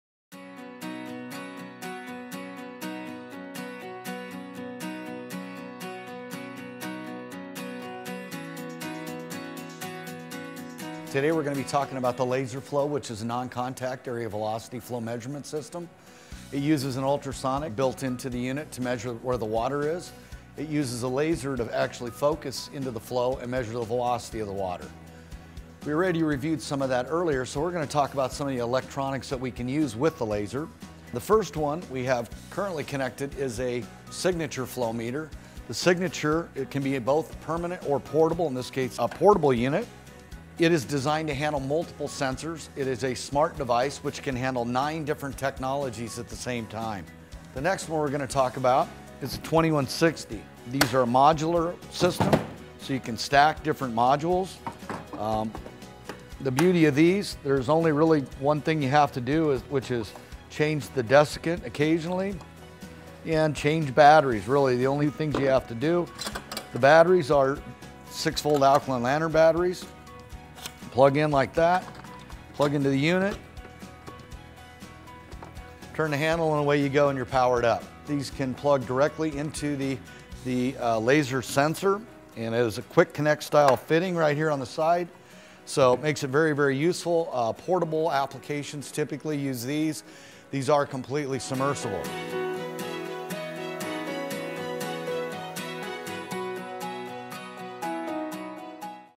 Lessons on LaserFlow | Episode 2 Compatible Technologies
Welcome back to another episode of Lessons on LaserFlow! In this episode, Teledyne ISCO's Rick Dey discusses the compatible flow meter technologies used with the LaserFlow.